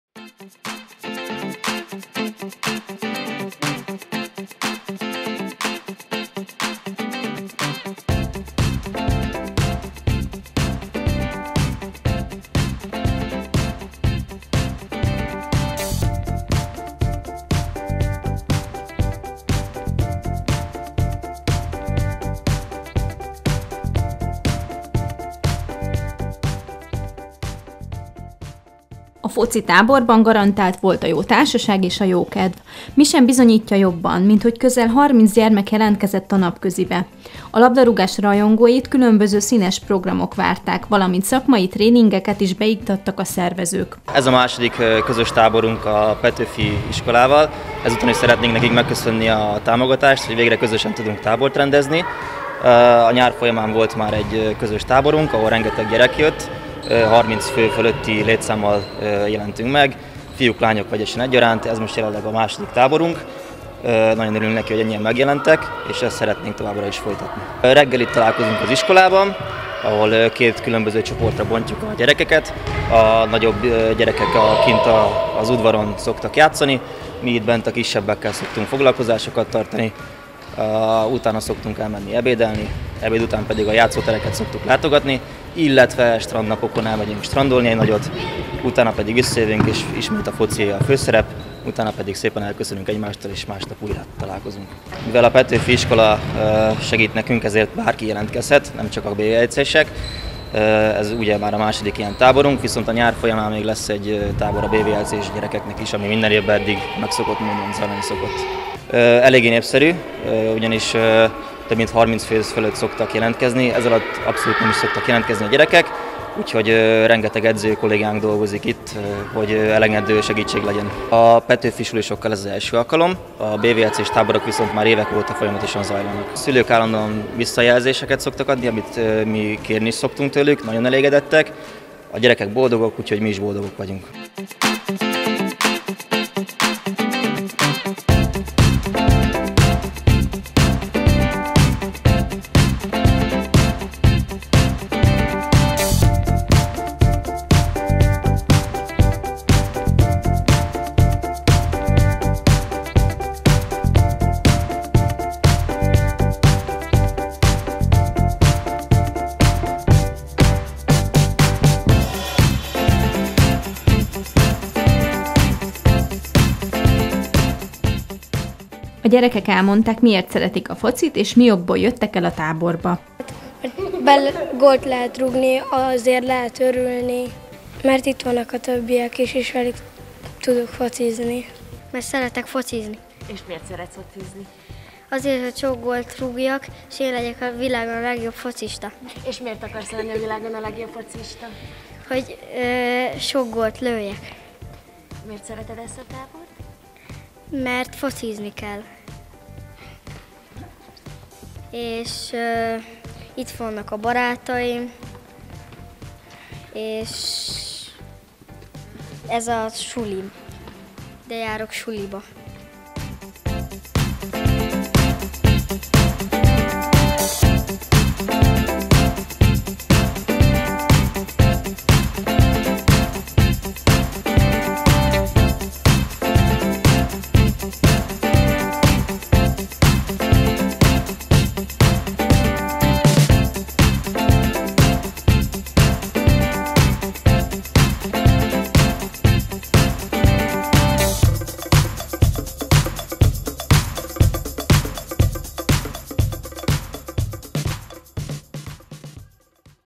Focitábor - különböző programok várták a focizni vágyókat Bonyhádon
A Bonyhád Völgységi Labdarúgó Club foci tábort szervezett a Petőfi Sándor Evangélikus Általános Iskolával együttműködve. A gyerekek a labdarúgáson kívül más szabad foglalkozáson is részt vettek.